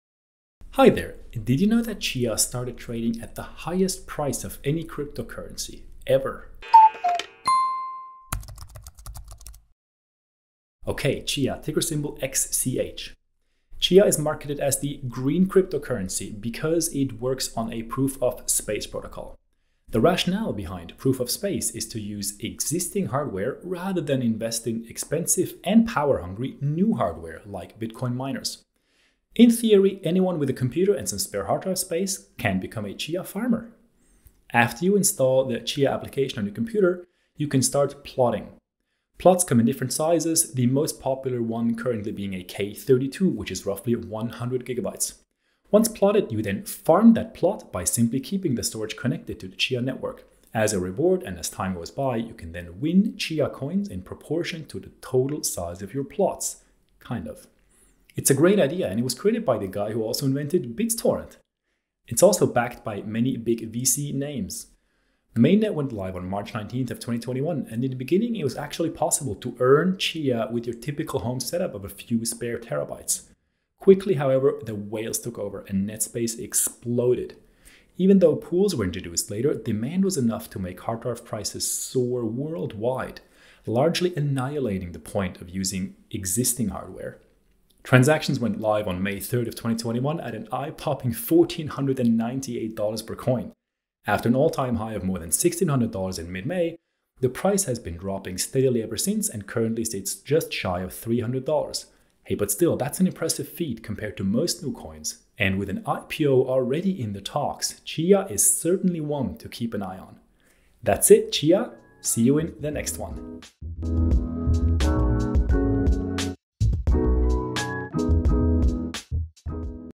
What is CHIA? Proof-Of-Space explained! [2min]
What is CHIA (XCH), the new "green" cryptocurrency? Is Proof-Of-Space (and time) really better than Proof-Of-Work, and Proof-Of-Stake? Find out in 2 Minutes!

Be social!

Show love?
BTC: bc1qw42uuw0gz4gxyy6g3e6t2yy4g4pddnxtqvh0my
ADA (BEP2): bnb1qjslklmnquesqe8n8ee997xmzyglsqq5nkjml5